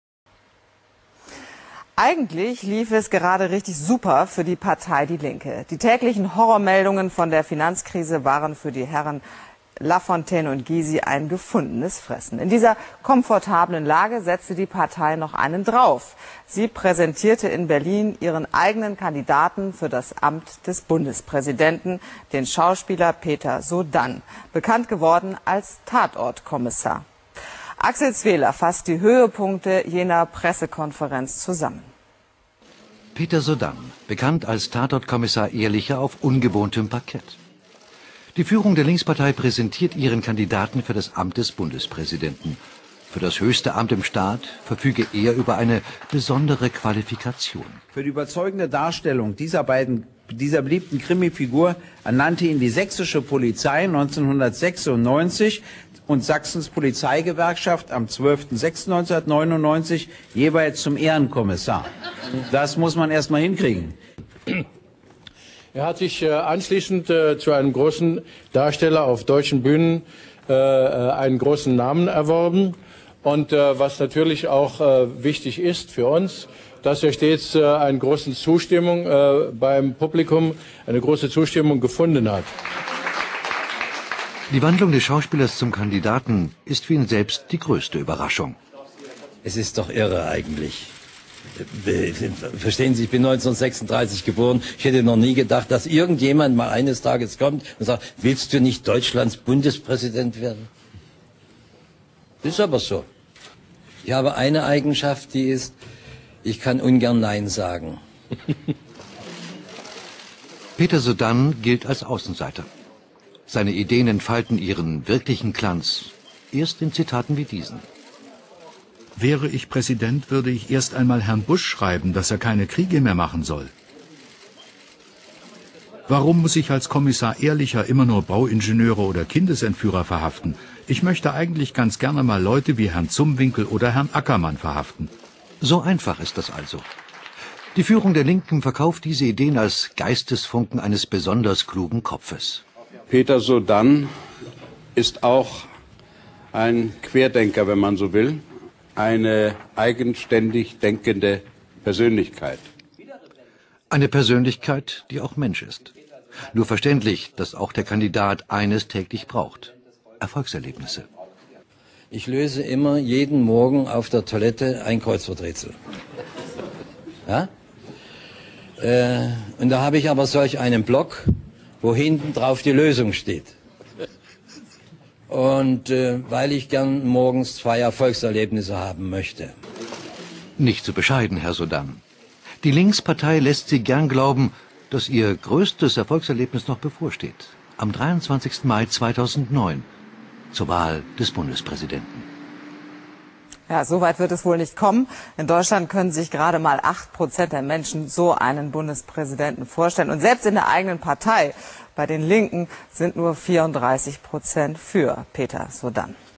Tatort-Kommissar auf Abwegen
Die Linke stellte im Oktober 2008 ihren Kandidaten für die Wahl zum Bundespräsidenten im darauffolgenden Jahr vor.